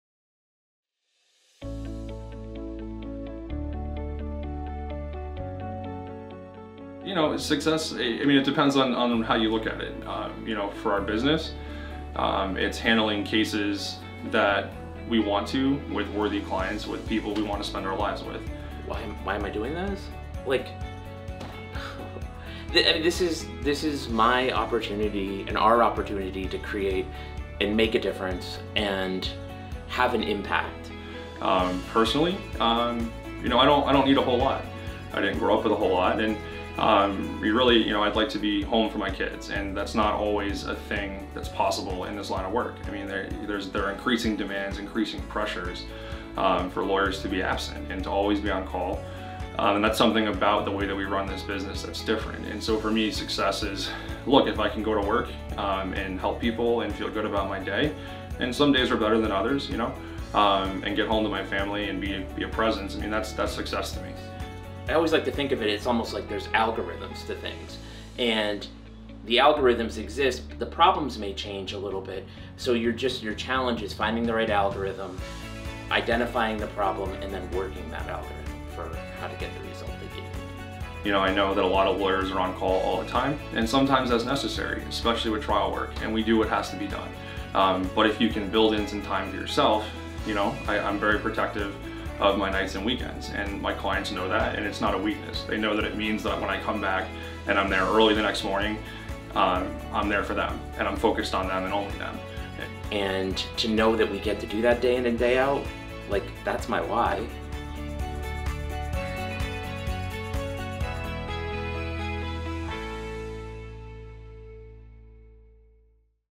Connecticut Trial Firm On Personal Success
How do you define success? Connecticut Personal Injury Attorneys Andrew Garza and Ryan McKeen weigh in on their personal definitions of success and how it drives Connecticut Trial Firm forward.